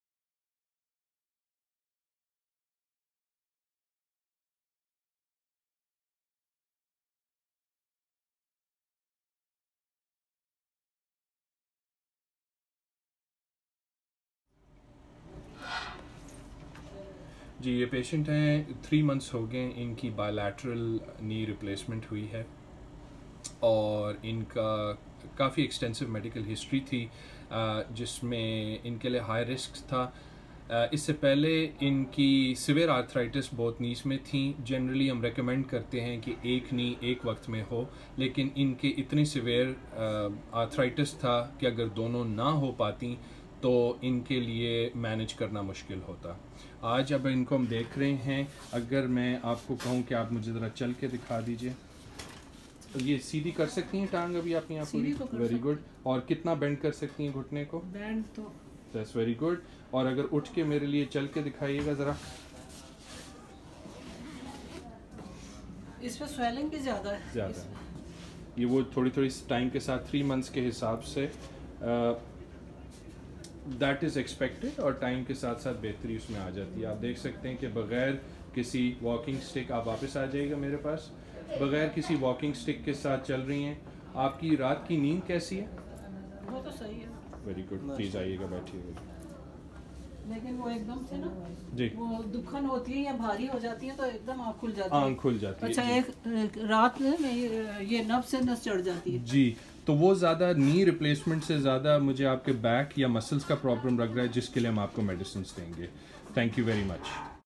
Bilateral Knee replacement by Mustafa Javed
Patients progress and feedback following knee replacement done on both knees in the same sitting. the patient is very pleased and happy with the results.